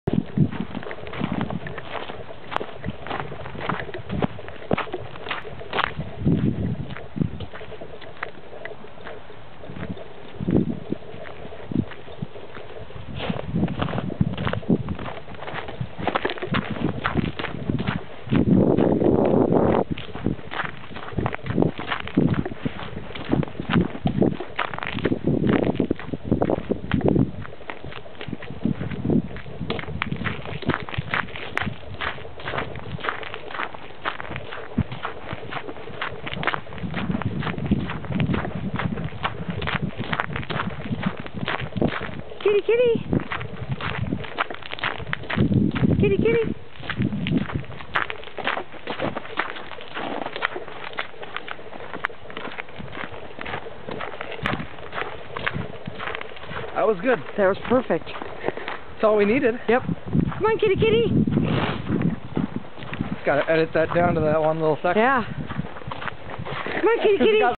KittieWalkies_12611.mov
Teazer the Tabby goes for walkies with her Dog Brother Dylan and Sister Dusty the Cairn Terriers, on a cold January afternoon.  Shot; 1/26/11